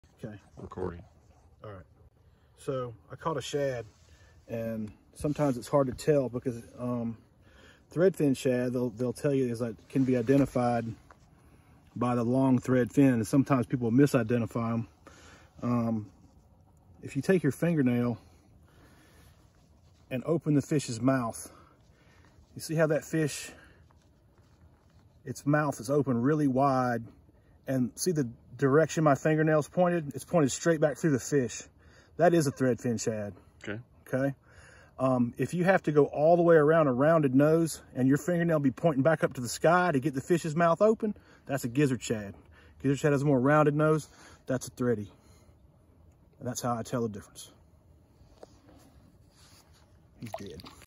How to tell the difference between threadfin & gizzard shad.
Snagged a shad, this is how the tell the difference between threadfin & gizzard shad. Gizzard shad also have the “threadfin” on the dorsal. That’s why people Mis ID them